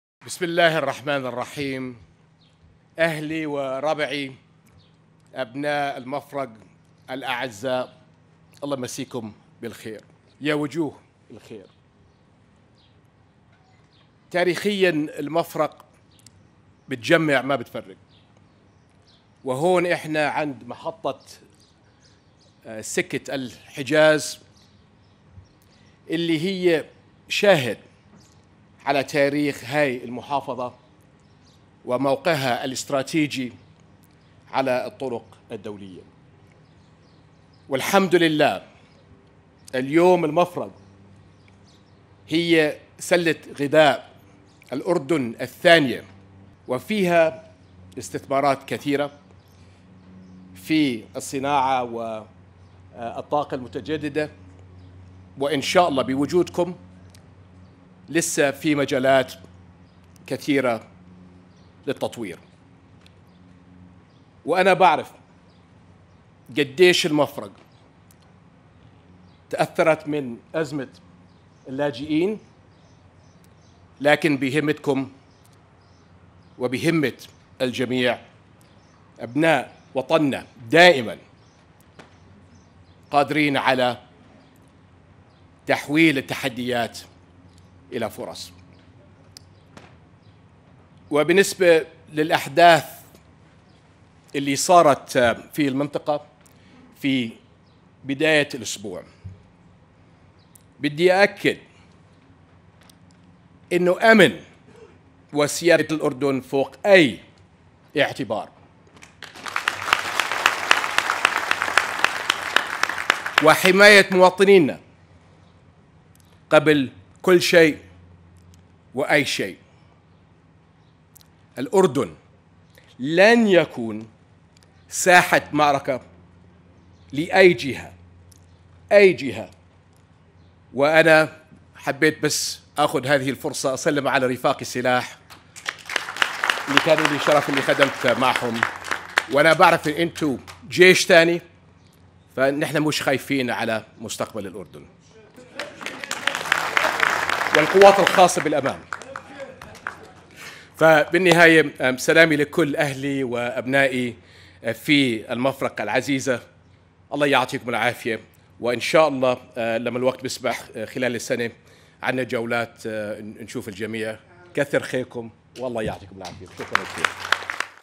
جلالة الملك: أمن الأردن وسيادته فوق كل اعتبار
جلالة الملك عبدالله الثاني خلال لقائه وجهاء وممثلين عن أبناء محافظة المفرق
- أمن الأردن وسيادته فوق كل اعتبار
- الأردن لن يكون ساحة معركة لأي جهة، وحماية مواطنينا قبل كل شيء